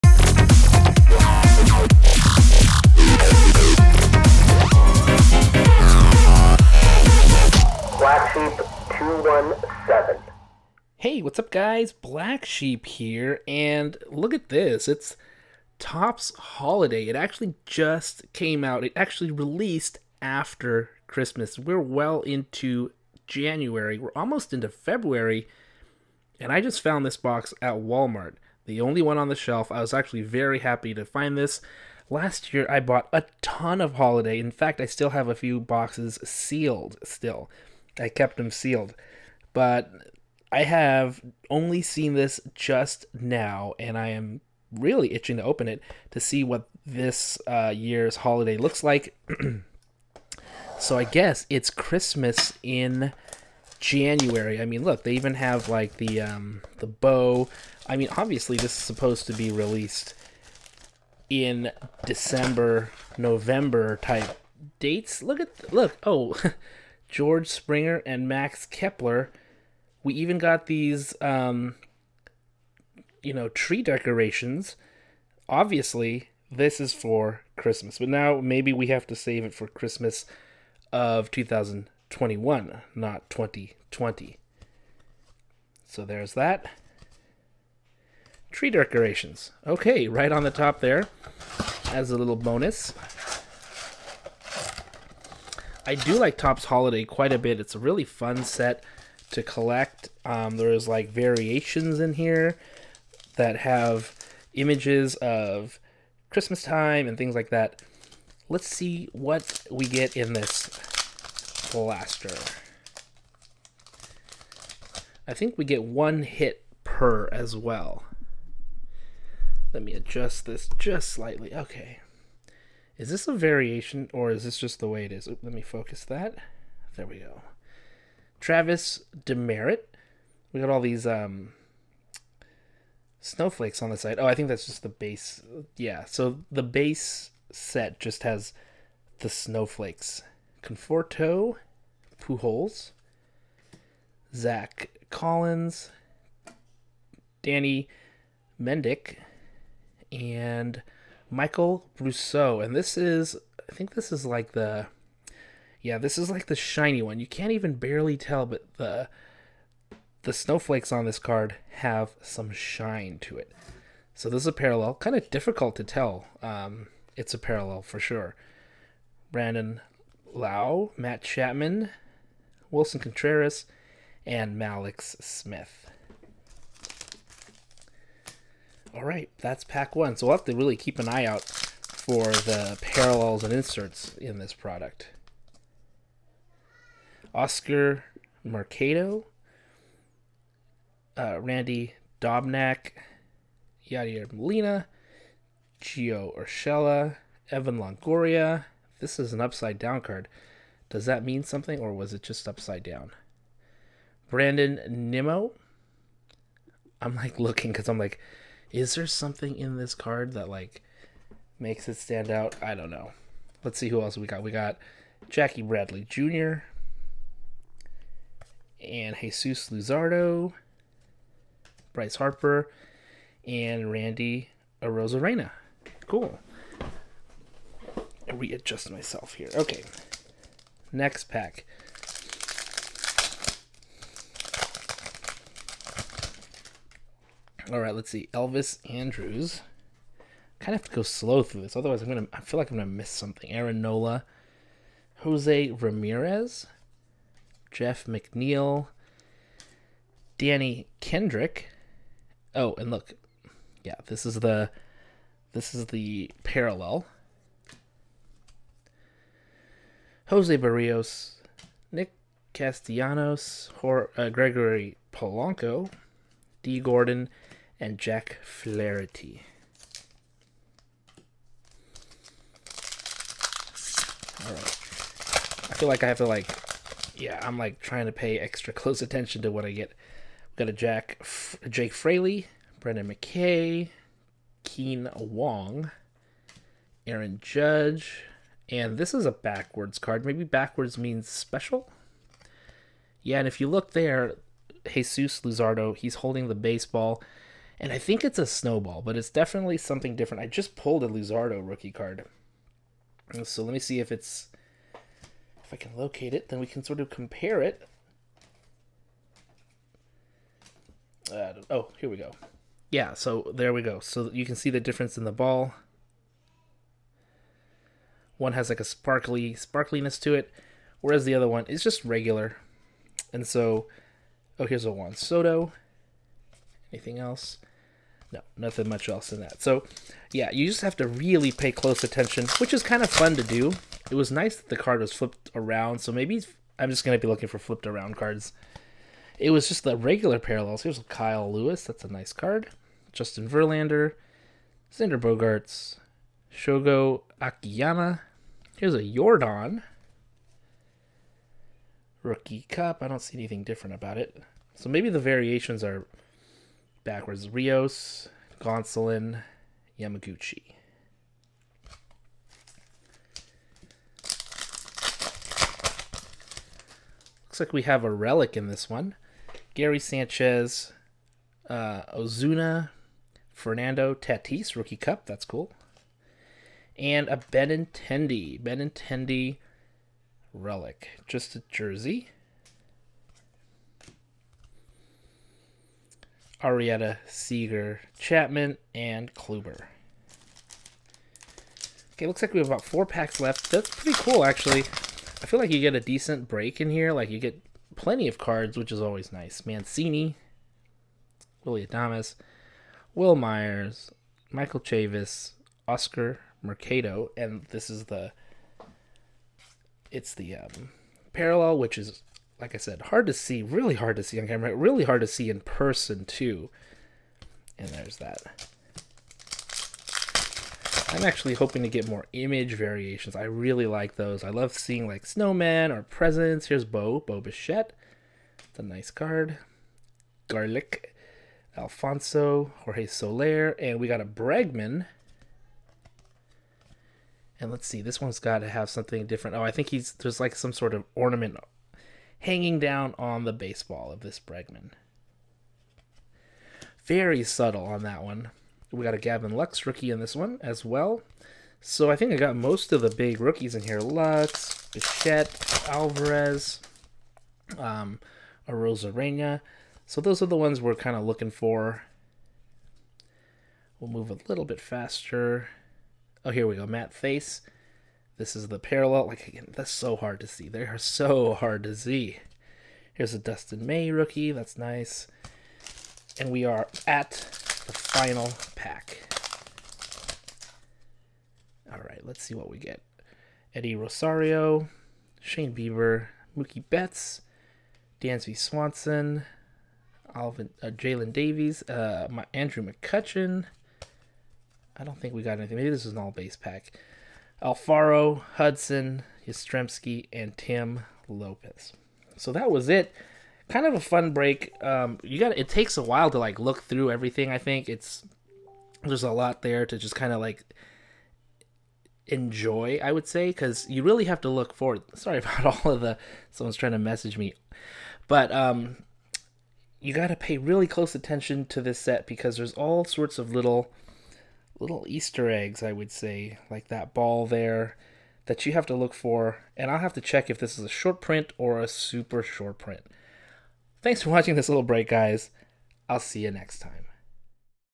2020 Topps Holiday Came Out Kinda LATE!!!
Well, at least it came out and I was able to find one box at my local Walmart.